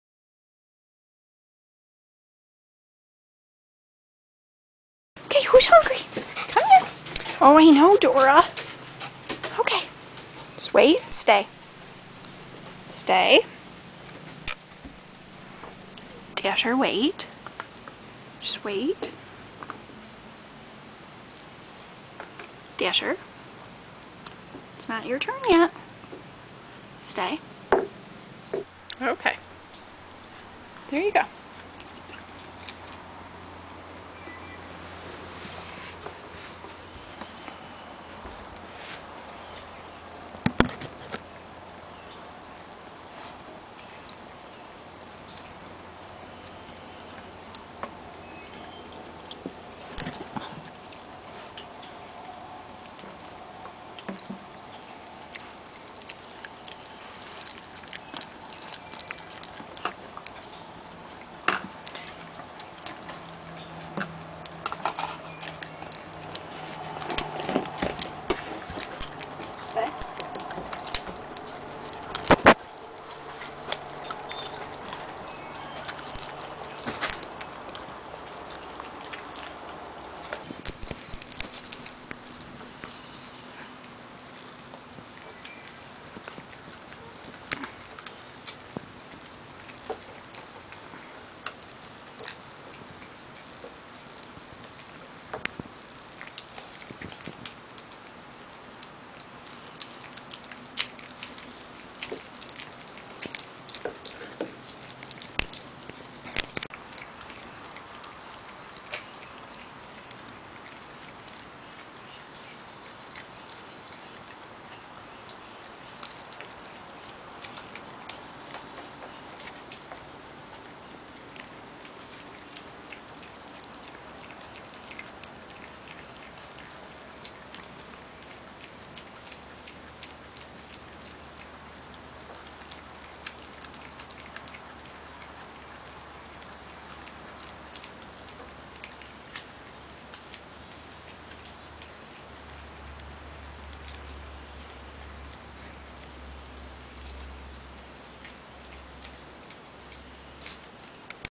Wolves
Belle loves RMB's.  Dash and Dora are still transitioning for now and only having them about once a week at this point.